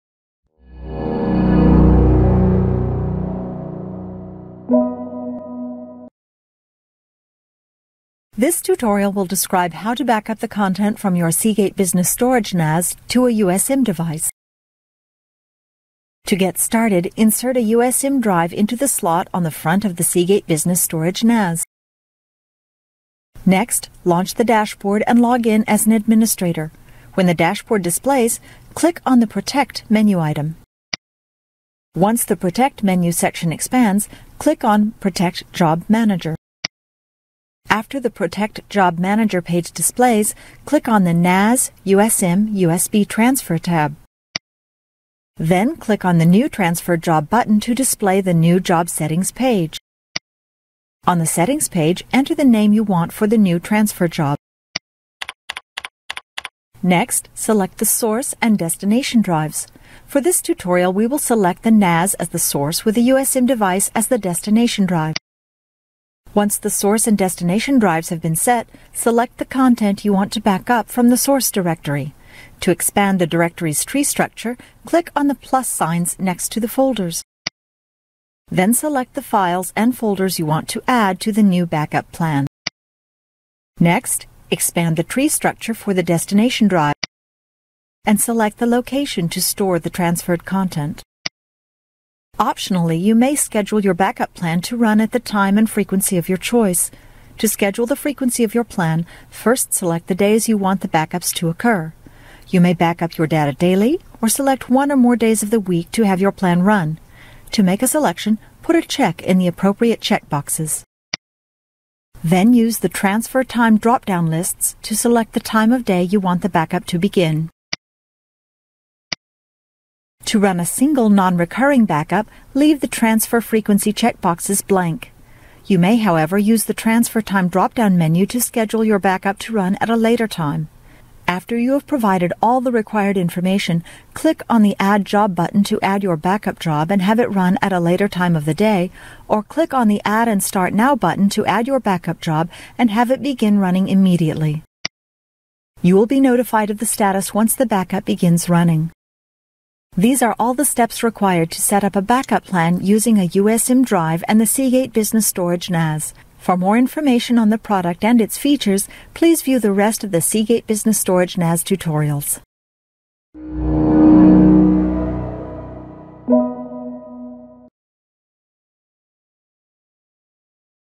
Business Storage NAS | How to Create a NAS to USM Backup Plan
This tutorial will show you how to back up your date from a Seagate Business Storage NAS device to a USM drive.